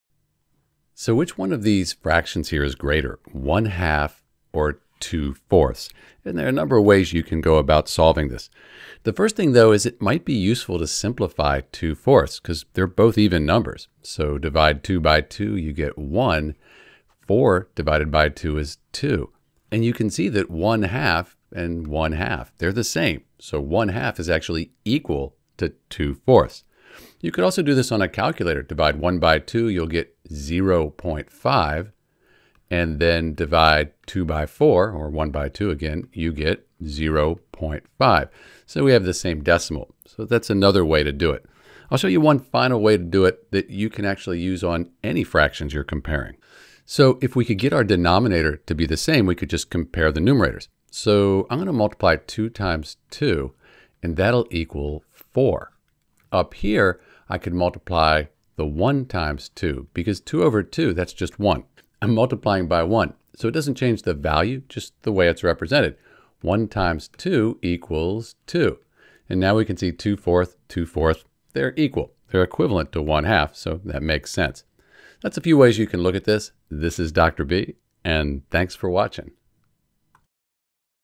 Which fraction is greater 1/2 or 2/4?
Second, we could also divide 1 by 2 and get 0.5. Then we divide 2 by 4 and we also get 0.5. This allows us to see that both fractions are equal.

In this case, both fractions are equal, so neither is greater than the other.